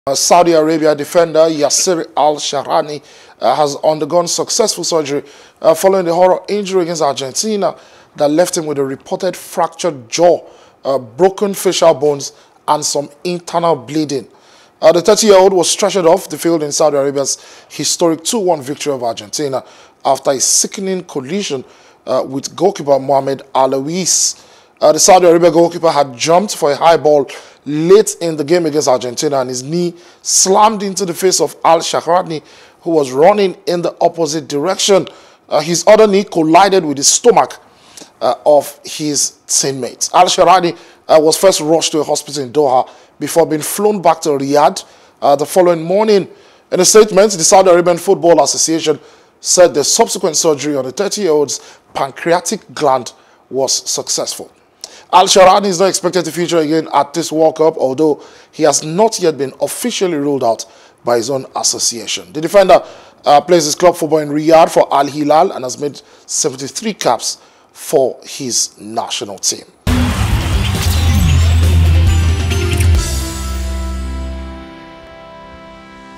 Qatar 2022: Saudi’s Al-Shahrani Undergoes Surgery After Horrific Collision | PLUS SPORTS
Welcome To Plus Sports

Saudi Arabia defender Yasser Al Shahrani has undergone successful surgery following the horror injury against Argentina that left him with a reported fractured jaw, broken facial bones, and internal bleeding.